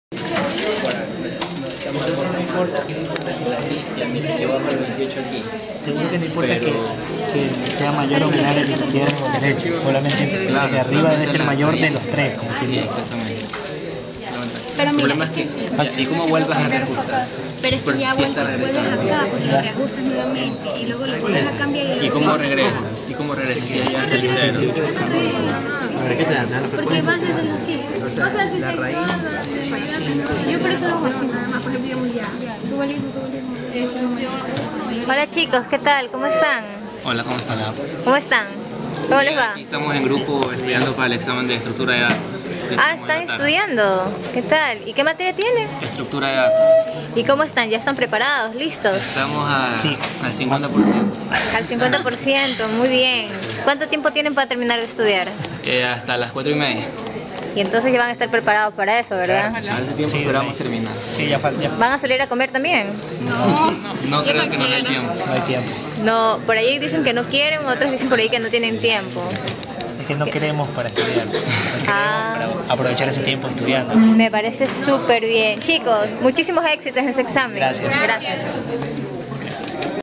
#4 Durante semana de exámenes en la ESPOL, ICQA, CSECT 2012.08.28
Se recogen vivencias de la semana de exámenes en los diferentes institutos, facultades, biblioteca y otros estamentos de la ESPOL.
Hay rostros complacidos por la preparación y la responsabilidad que conlleva  al momento de mostrar los conocimientos adquiridos durante el semestre.